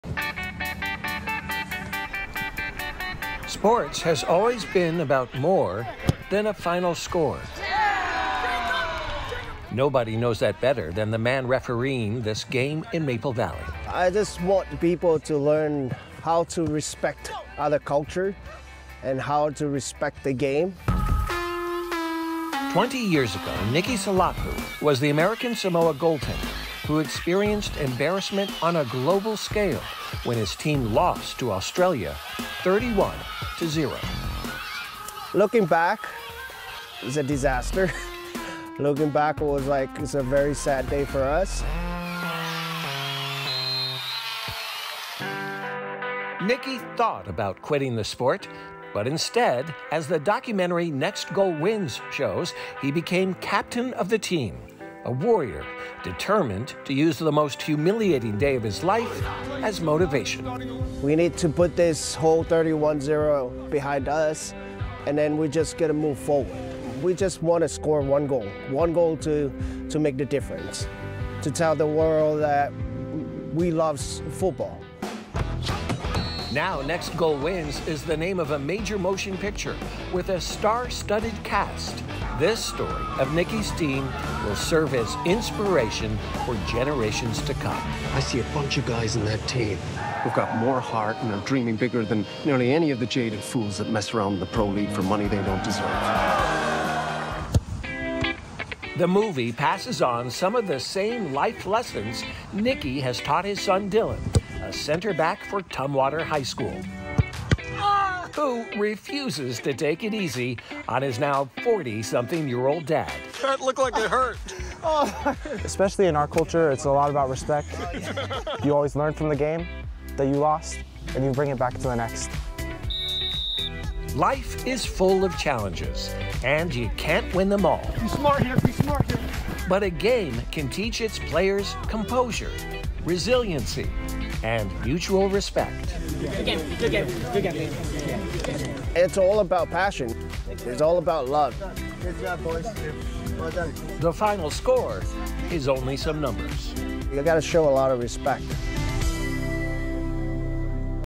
The most humiliating day of his life inspired hit comedy 'Next Goal Wins'
Olympia's Nicky Salapu can teach us all how to put our worst day behind us.